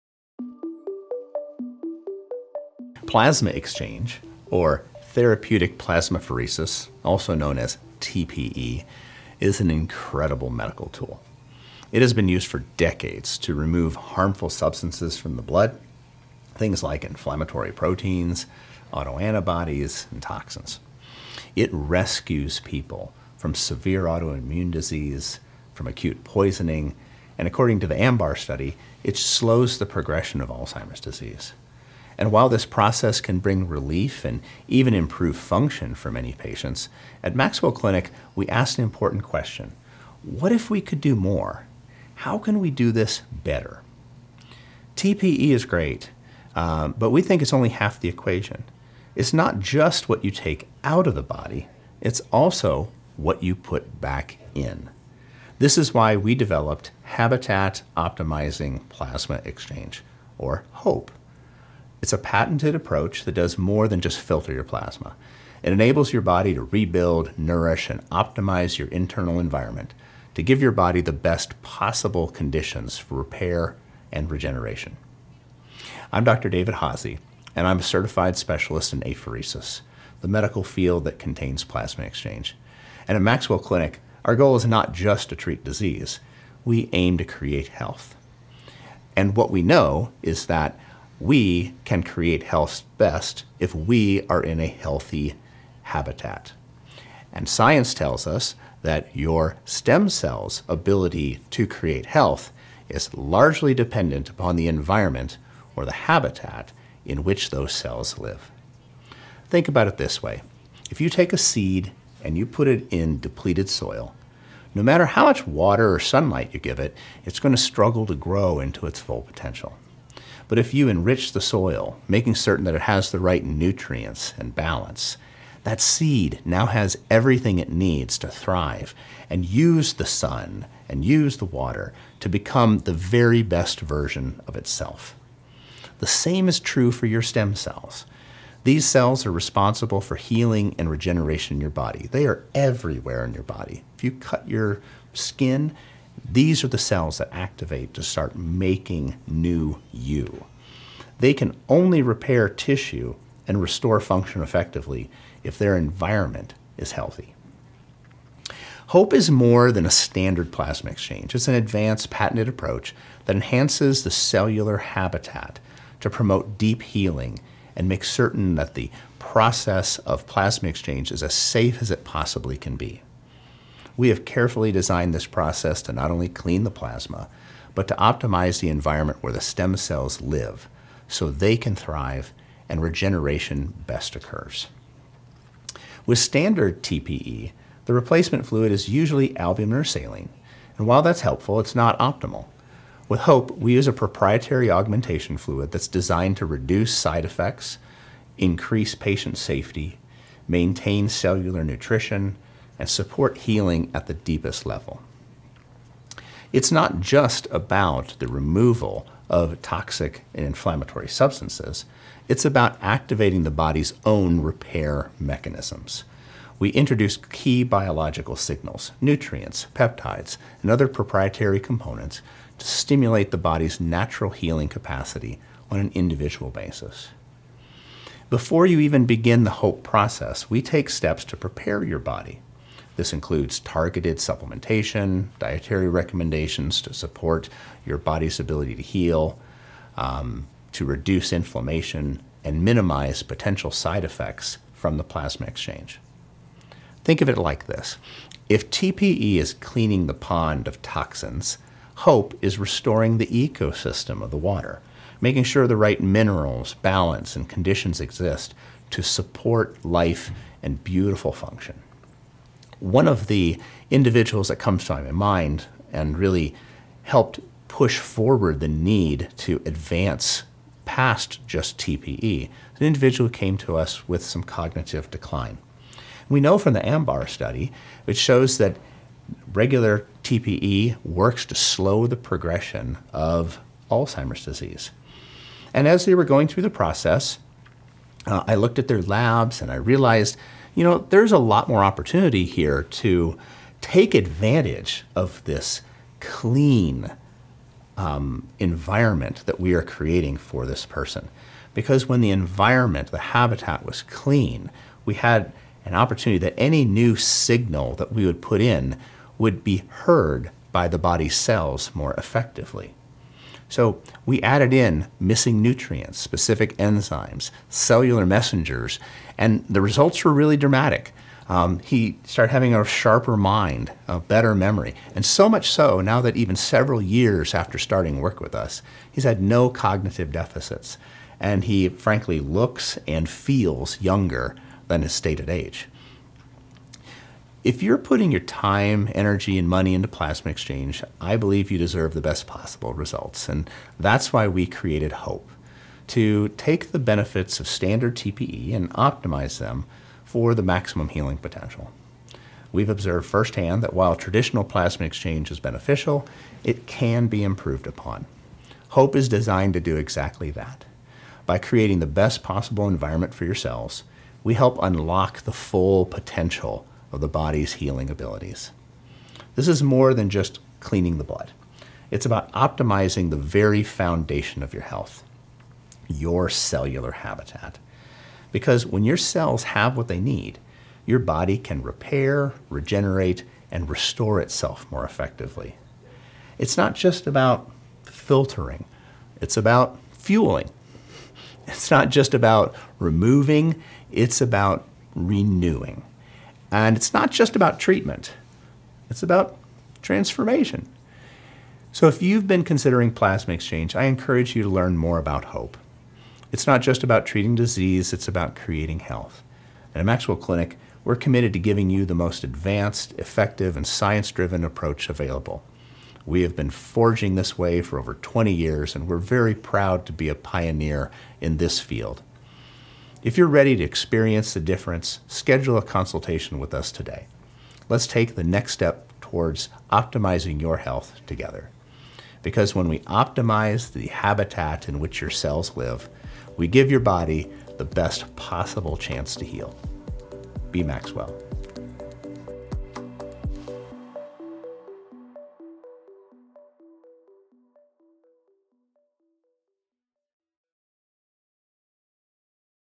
Therapeutic Plasma Exchange vs Habitat Optimizing Plasma Exchange | David Haase, MD
Why settle for standard plasma exchange when you can optimize your body’s ability to heal?

Traditional Therapeutic Plasma Exchange (TPE) removes harmful substances like toxins, inflammatory proteins, and autoantibodies—but at MaxWell Clinic, we asked: What if we could do more?

Introducing Habitat Optimizing Plasma Exchange (HOPE)—a patented, next-generation approach that goes beyond standard TPE to enhance cellular regeneration, optimize the healing environment, and support long-term health.

In this video, Dr. David Haase, MD, explains:
 ✅ How HOPE improves upon traditional TPE for better results
 ✅ The key differences between HOPE & standard plasma exchange
 ✅ How a healthier cellular environment fuels regeneration & longevity
 ✅ A real patient success story demonstrating the dramatic benefits of HOPE

💡 HOPE isn’t just about filtering—it’s about fueling. It’s not just about removing—it’s about renewing.
If you’re considering plasma exchange, make sure you’re getting the most out of it. Discover how HOPE can optimize your body’s ability to heal today.

Your health is the most valuable asset you have—don't wait and take control of your health today!

00:00 What is Plasma Exchange?
00:41 Why Standard TPE Isn’t Enough
01:22 Introducing HOPE™: A New Approach
02:42 How HOPE™ Optimizes Your Cellular Environment
04:42 Real Patient Success Story – How HOPE™ transformed cognitive health and long-term well-being
06:41 Why HOPE™ is the Future of Plasma Exchange
07:12 Take the Next Step